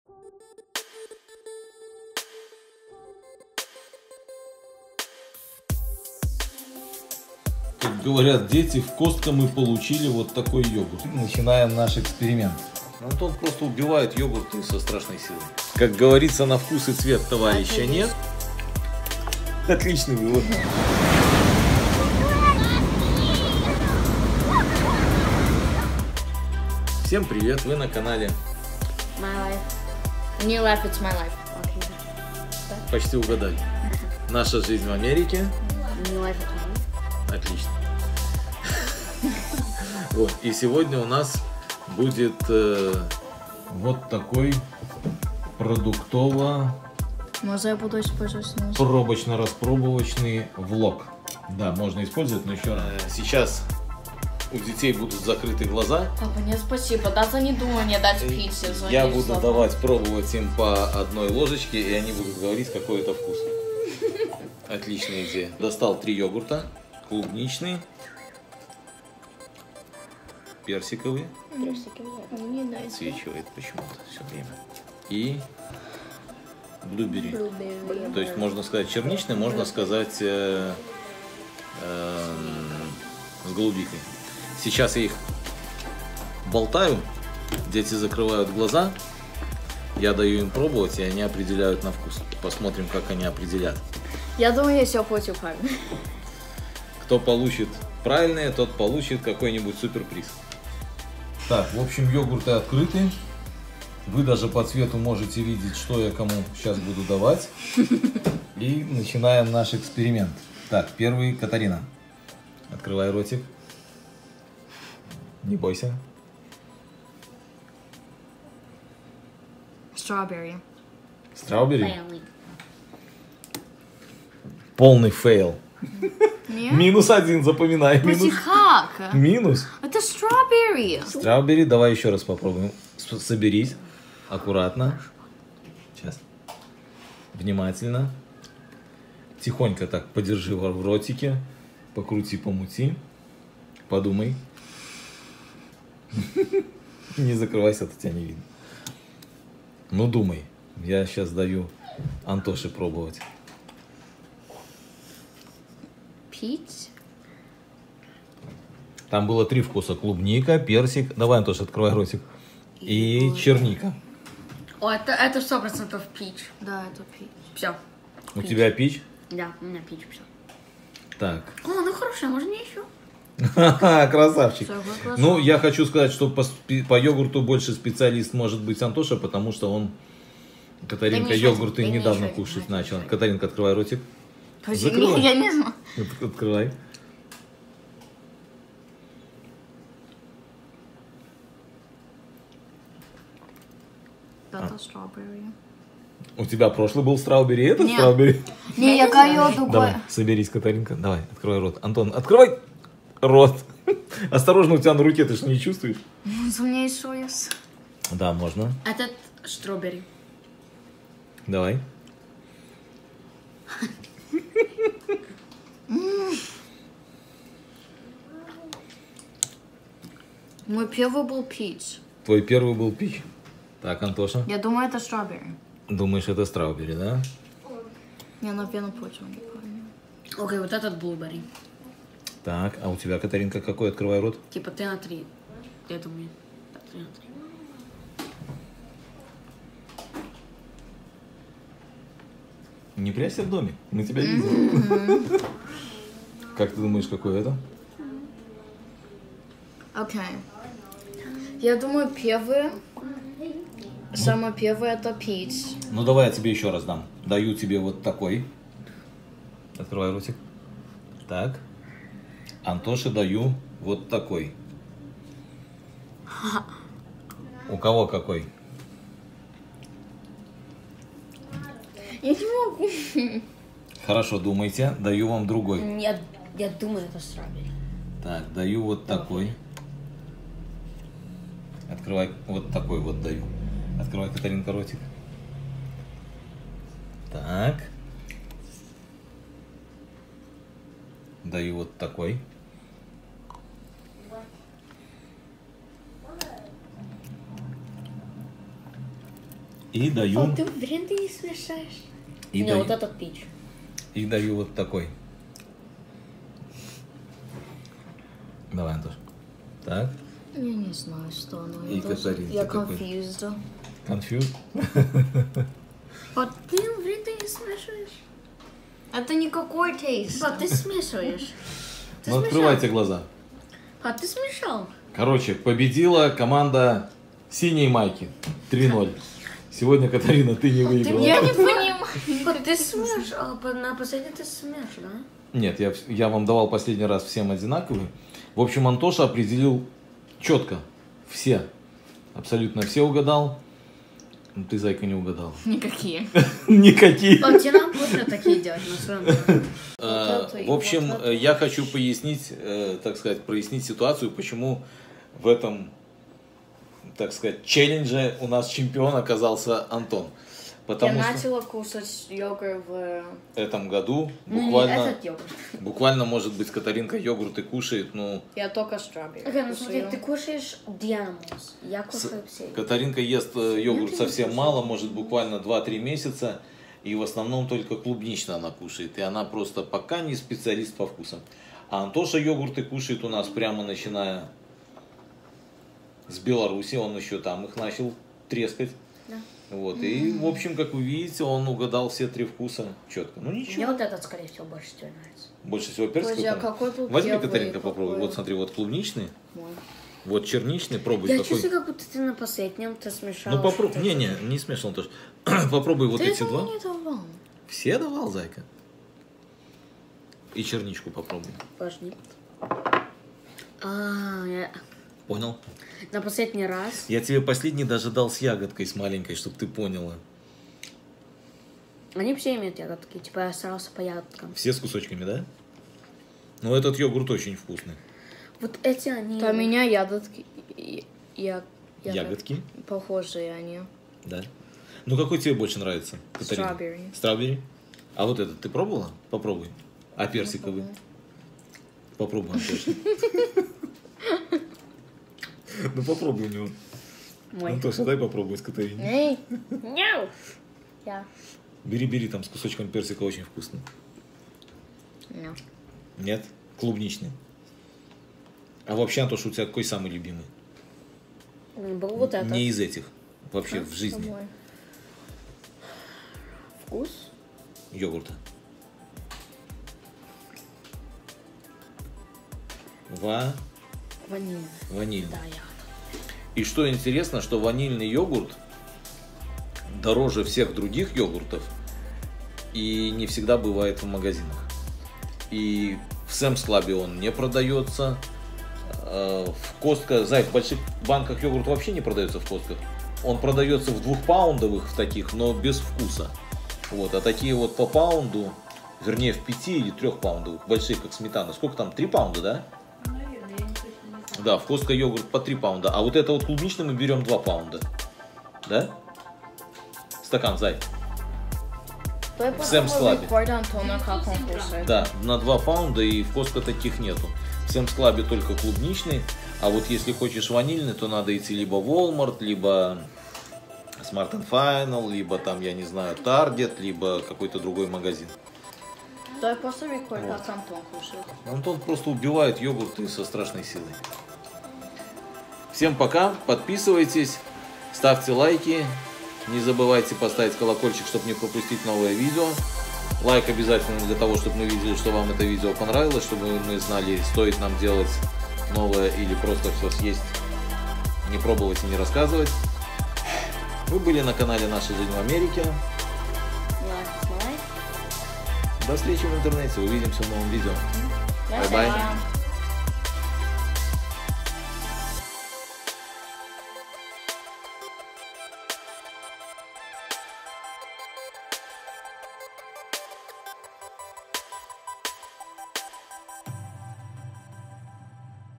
Йогурт челлендж в Америке. Жизнь в США. Дети в Америке. Продукты в США. Сладкая жизнь в Америке!
Йогурт челлендж в Америке. Распаковываем, изучаем состав и пробуем йогурт Yoplait из Costco в США. Что кушают дети в Америке. Продукты в США. Сладкая жизнь в Америке!